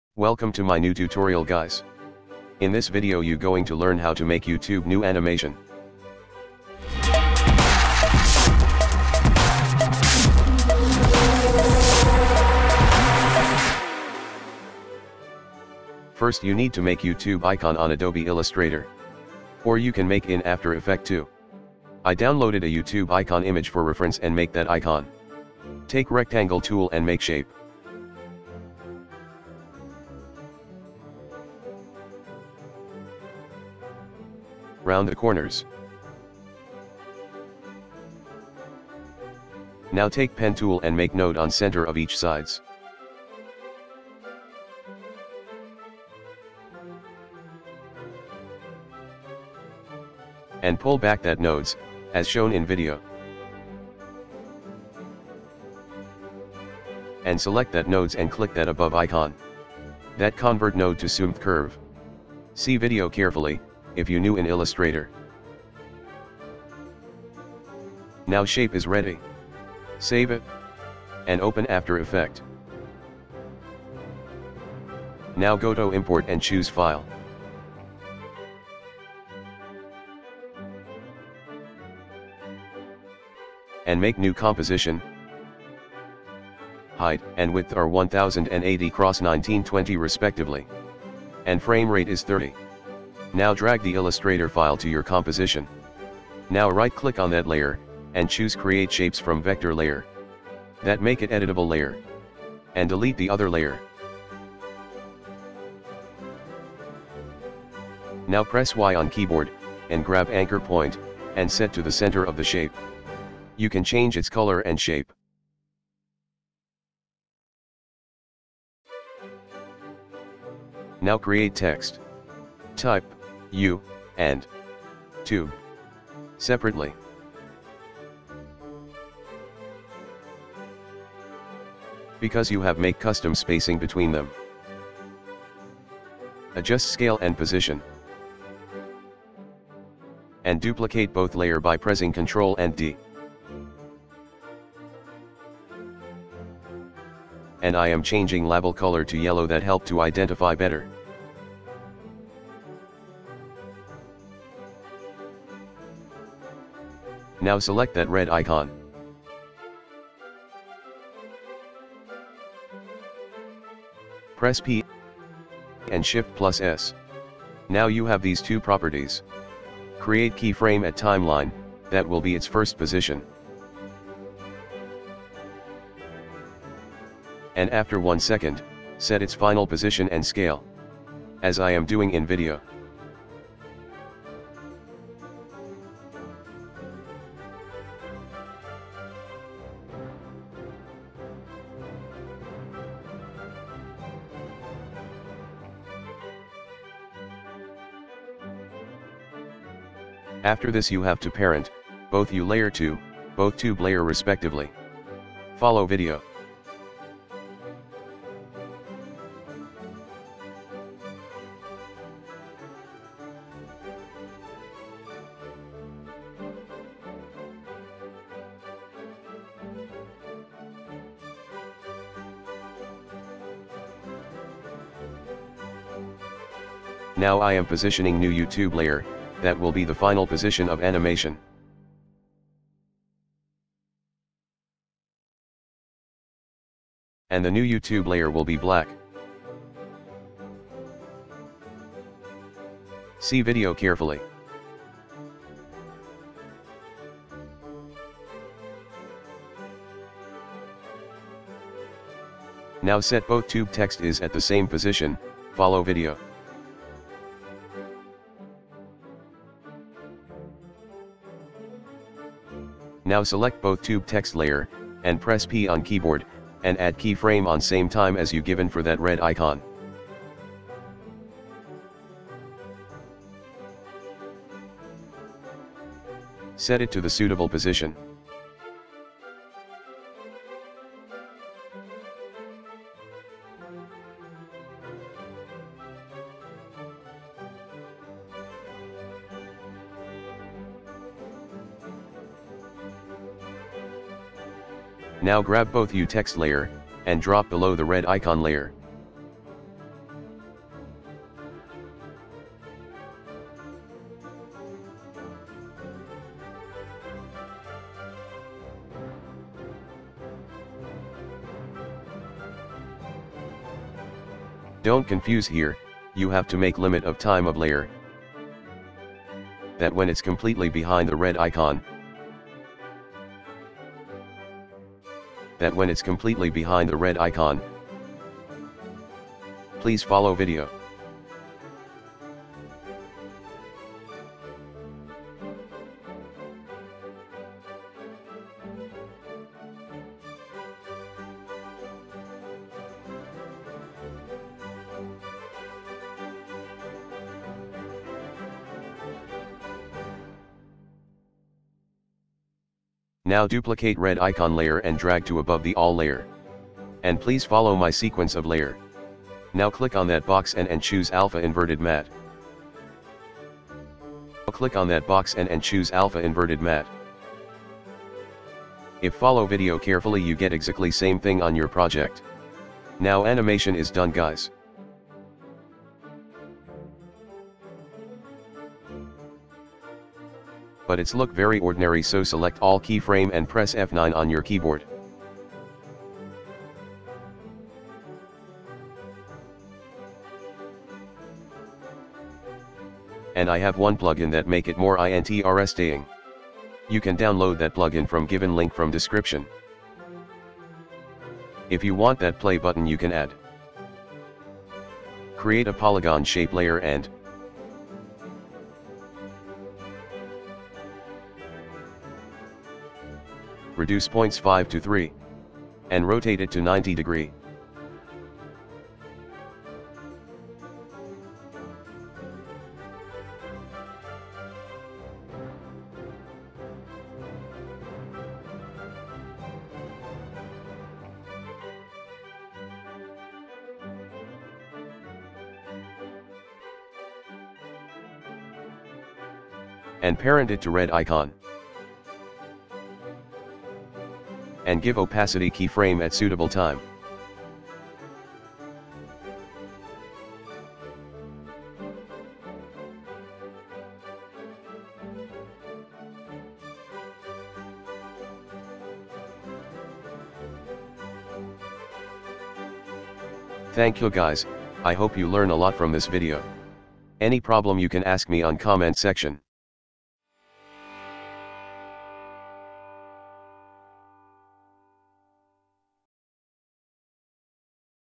How to make YouTube new logo animation 2017 | after effects | Tutorial 16
at
5:14 press shft+D - Trim video
6:32 press U - shows all property do you used,

Watch my more videos related to animation and illustration